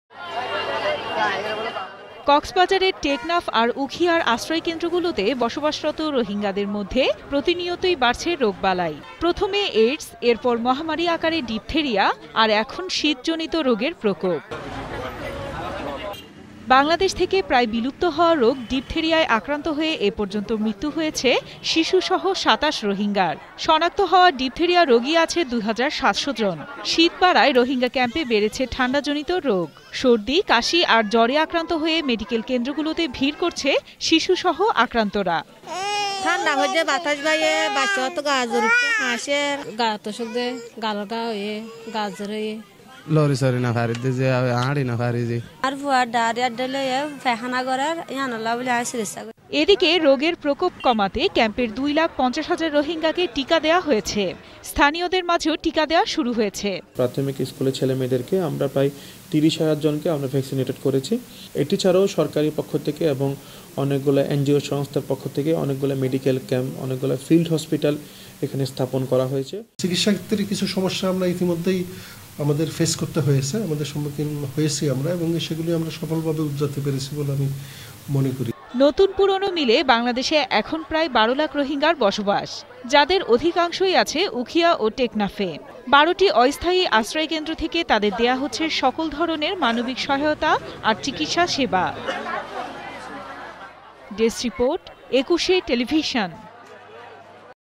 রোহিঙ্গা ক্যাম্পে চিকিৎসা দিচ্ছে স্থানীয় মেডিকেল ক্যাম্পগুলো | ETV News
রোহিঙ্গা ক্যাম্পে বাড়ছে বিভিন্ন রোগে আক্রান্ত রোগির সংখ্যা। আগে ছিলো এইডস আর ডিপথেরিয়া, এখন শীত বাড়ায় ঠান্ডাজনিত রোগে আক্রান্ত হচ্ছে রোহিঙ্গা শিশু আর বয়স্করা। শিশুদের টিকাদানের পাশাপাশি শীতজনিত রোগের চিকিৎসা দিচ্ছে স্থানীয় মেডিকেল ক্যাম্পগুলো। তবে রোগীর চাপ সামলাতে হিমশিম খেতে হচ্ছে তাদের।
কক্সবাজারের টেকনাফ আর উখিয়ার আশ্রয়কেন্দ্র গুলোতে বসবাসরত রোহিঙ্গাদের মধ্যে প্রতিনিয়তই বাড়ছে রোগ বালাই। প্রথমে এইডস, এর পর মহামারি আকারে ডিপথেরিয়া, আর এখন শীত জনিত রোগের প্রকোপ।
বাংলাদেশ থেকে প্রায় বিলুপ্ত হওয়া রোগ ডিপথেরিয়ায় আক্রান্ত হয়ে এপর্যন্ত মৃত্যু হয়েছে শিশুসহ ২৭ রোহিঙ্গার। সনাক্ত হওয়া ডিপথেরিয়া রোগী আছে ২হাজার ৭শ’জন। শীত বাড়ায় রোহিঙ্গা ক্যাম্পে বেড়েছে ঠান্ডাজনিত রোগ। সর্দি, কাশি আর জ্বরে আক্রান্ত  হয়ে মেডিকেল কেন্দ্র গুলোতে ভীড় করছে শিশুসহ আক্রান্তরা ।
এদিকে রোগের প্রকোপ কমাতে ক্যাম্পের দুই লাখ ৫০হাজার রোহিঙ্গাকে টিকা দেয়া হয়েছে। স্থানীয়দের মাঝেও টিকা দেয়া শুরু হয়েছে।
নতুন পুরনো মিলে বাংলাদেশে এখন প্রায় ১২লাখ রোহিঙ্গার বসবাস। যাদের অধিকাংশই আছে উখিয়া ও টেকনাফে। ১২ টি অস্থায়ী আশ্রয় কেন্দ্র থেকে তাদের দেয়া হচ্ছে সকল ধরনের মানবিক সহায়তা আর চিকিৎসাসেবা।